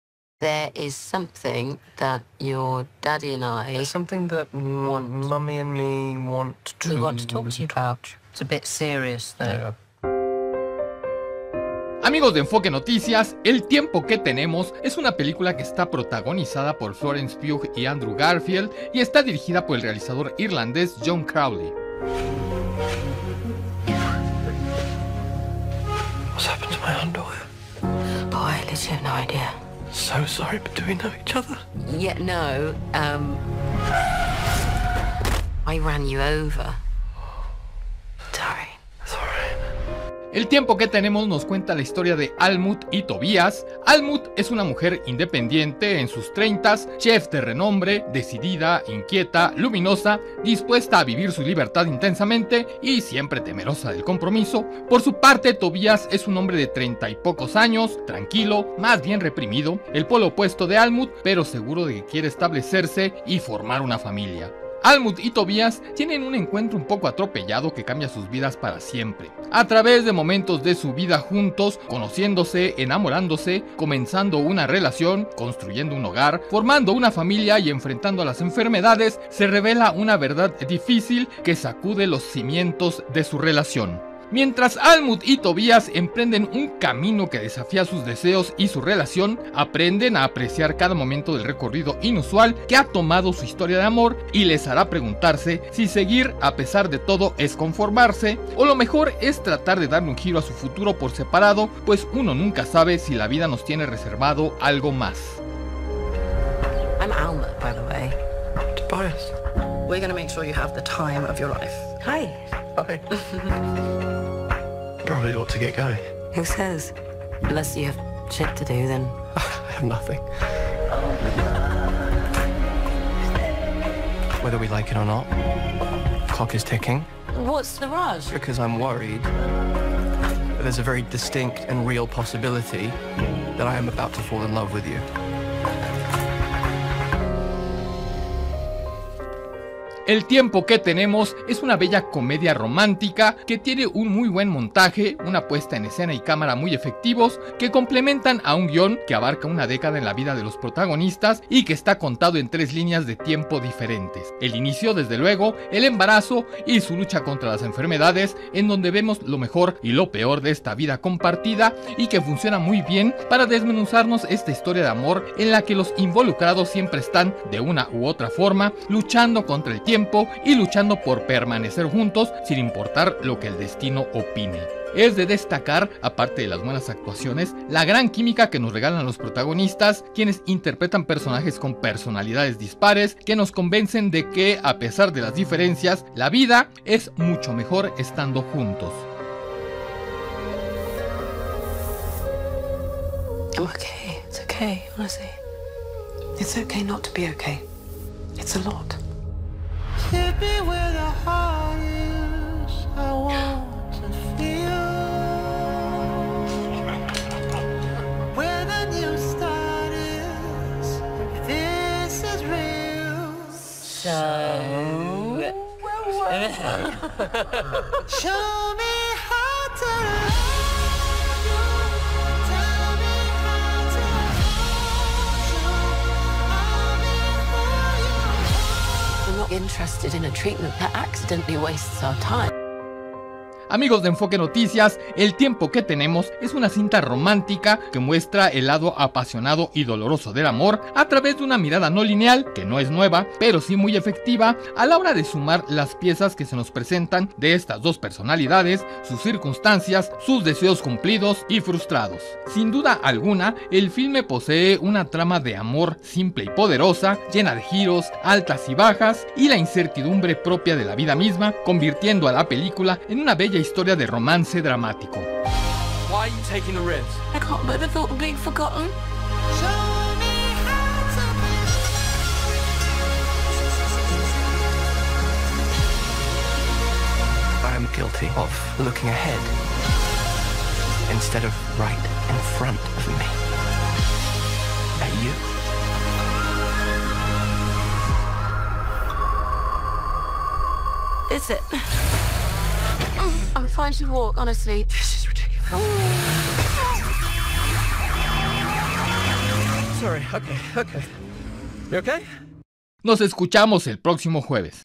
Enfoque al Séptimo Arte: "EL TIEMPO QUE TENEMOS"
“El Tiempo que Tenemos” es una bella comedia romántica, que tiene un muy buen montaje, puesta en cámara y en escena, que complementan a un guión que abarca una década en la vida de sus protagonistas.

La trama está contada en tres líneas de tiempo diferentes, el inicio, desde luego, el embarazo y su lucha contra las enfermedades.

En estos tres fragmentos de su relación vemos lo mejor y lo peor de esta vida compartida.